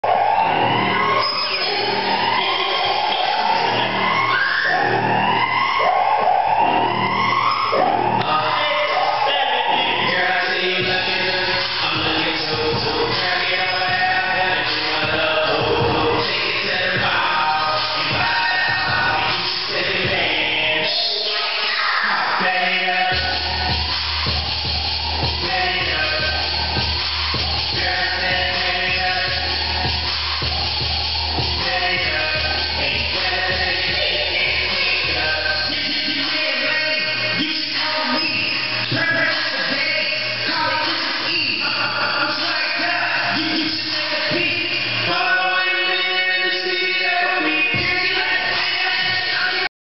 Rollins Sisters and Jason Croom
Hannah (age 11) and Emily (age 8) Rollins performing in groups with Swagger Crew's Jason Croom during one of his masterclasses in Atlanta!!!